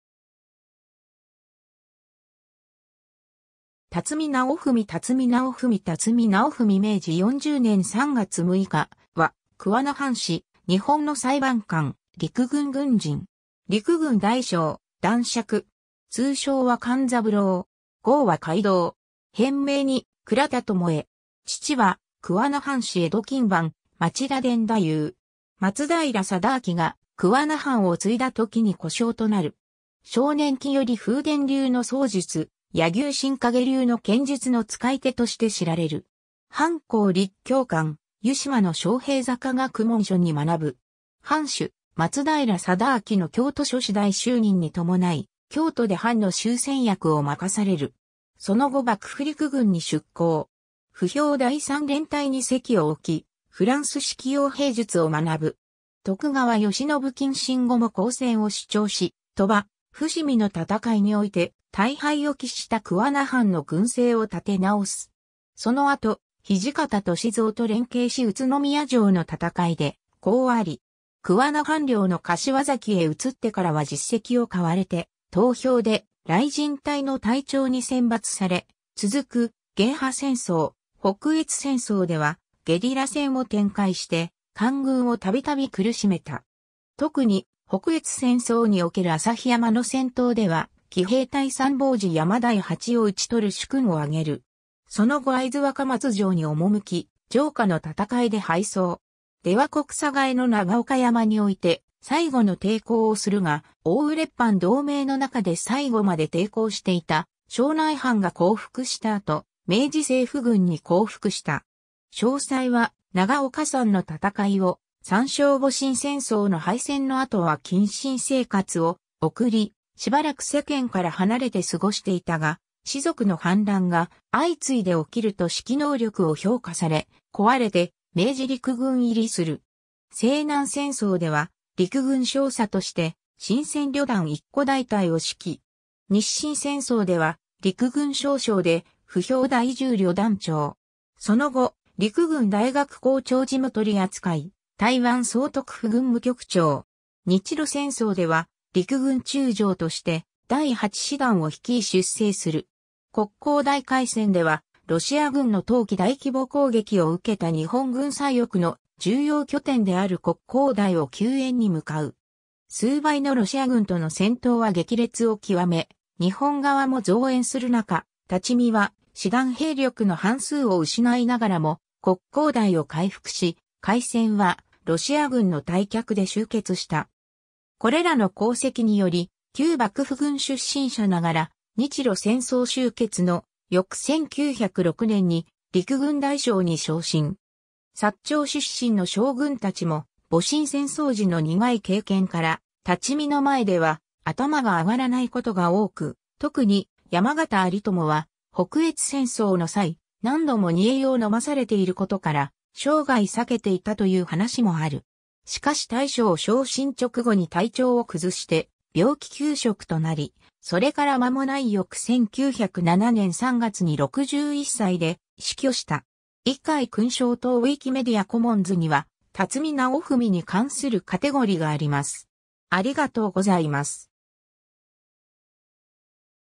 立見尚文
立見尚文 立見尚文 立見 尚文（たつみ なおふみ/なおぶみ、弘化2年7月19日（1845年8月21日） - 明治40年（1907年）3月6日）は桑名藩士、日本の裁判官・陸軍軍人。
陸軍大将、男爵。
通称は鑑三郎。
号は快堂。
変名に倉田巴。
父は桑名藩士江戸勤番・町田伝太夫。
 松平定敬が桑名藩を継いだときに小姓となる。
少年期より風伝流の槍術、柳生新陰流の剣術の使い手として知られる。
藩校立教館、湯島の昌平坂学問所に学ぶ。
 藩主松平定敬の京都所司代就任に伴い京都で藩の周旋役を任される。
その後幕府陸軍に出向。
歩兵第3連隊に籍を置きフランス式用兵術を学ぶ。
 徳川慶喜謹慎後も抗戦を主張し、鳥羽・伏見の戦いにおいて大敗を喫した桑名藩の軍制を立て直す。
その後土方歳三と連繋し宇都宮城の戦いで功あり。
 桑名藩領の柏崎へ移ってからは実績を買われて投票で雷神隊の隊長に選抜され、続く鯨波戦争・北越戦争ではゲリラ戦を展開して官軍を度々苦しめた。
特に北越戦争における朝日山の戦闘では、奇兵隊参謀時山直八を討ち取る殊勲を挙げる。
その後会津若松城に赴き、城下の戦いで敗走。
出羽国寒河江の長岡山において最後の抵抗をするが、奥羽列藩同盟の中で最後まで抵抗していた庄内藩が降伏した後、明治政府軍に降伏した。
 詳細は「長岡山の戦い」を参照 戊辰戦争の敗戦の後は謹慎生活を送り、しばらく世間から離れて過ごしていたが、士族の反乱が相次いで起きると指揮能力を評価され、請われて明治陸軍入りする。
西南戦争では陸軍少佐として新撰旅団一個大隊を指揮。
日清戦争では陸軍少将で歩兵第10旅団長。
その後、陸軍大学校長事務取扱、台湾総督府軍務局長。
 日露戦争では陸軍中将として第8師団を率い出征する。
黒溝台会戦では、ロシア軍の冬季大規模攻撃を受けた日本軍左翼の重要拠点である黒溝台を救援に向かう。
数倍のロシア軍との戦闘は激烈を極め、日本側も増援する中、立見は師団兵力の半数を失いながらも黒溝台を回復し、会戦はロシア軍の退却で終結した。
 これらの功績により、旧幕府軍出身者ながら日露戦争終結の翌1906年に陸軍大将に昇進。
薩長出身の将軍たちも、戊辰戦争時の苦い経験から立見の前では頭が上がらないことが多く、特に山縣有朋は北越戦争の際、何度も煮え湯を飲まされている（多大な損害を与えられ、逃亡さえして、味方にも相当な非難を浴びた）ことから、生涯避けていたという話もある。
しかし大将昇進直後に体調を崩して病気休職となり、それから間もない翌1907年3月に61歳で死去した。
 位階 勲章等 ウィキメディア・コモンズには、立見尚文に関するカテゴリがあります。